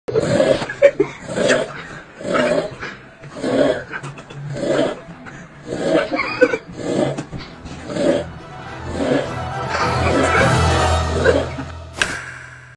Neuvillette snoring every 0.01 Second
Neuvillette Snoring every 0.1 Seconds...
[Improved visuals and animation]

- Credits -
Models : Hoyoverse
Shader: Festvity, Bonny
Audio : Instagram video (let me know if you have the original so i can update the link here)
Animation / rendering : me

- Software used (most of the time) -
Blender
After Effects